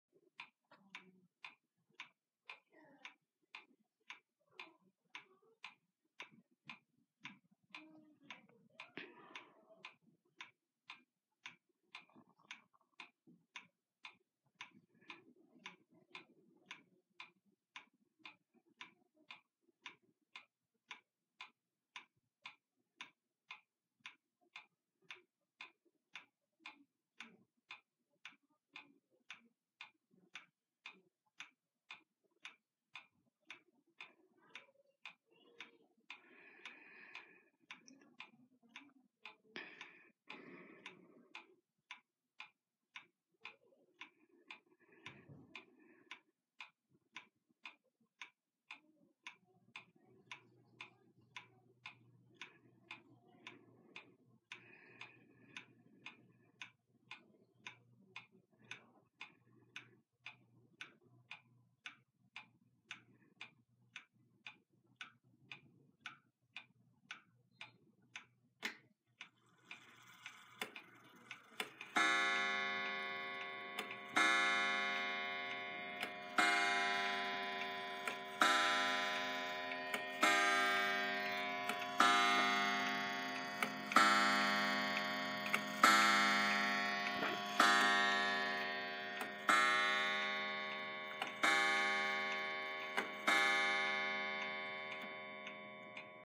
Centurion 35 day wall clock striking noon.
I got this clock In August, 2021.  The clock runs fast for some reason, even when I adjust the pendulum.  It's a really pleasant clock, and the strike is beautiful.  There actually was a little card that read "Centurion" on it, but it fell off.